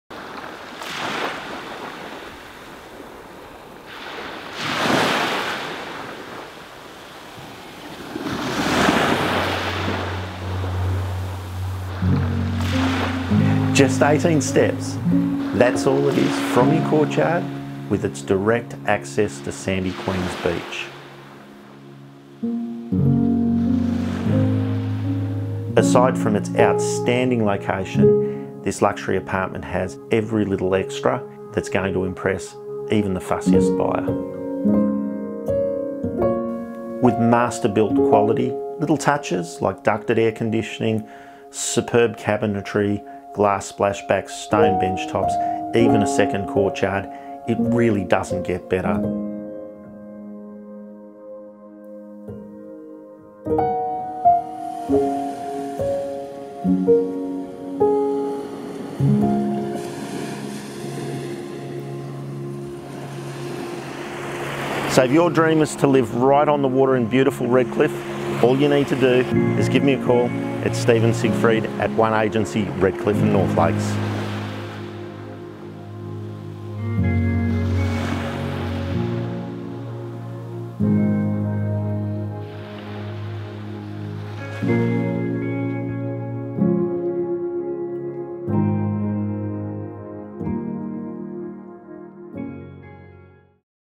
18 Steps to sandy Queens Beach Redcliffe
The very best absolute beachfront unit available.
One of the very best in Queensland.
Minutes to Redcliffe and Scarborough in the northern suburbs of Brisbane.
Contract Stephan Siegfried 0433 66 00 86
One Agency Redcliffe - North Lakes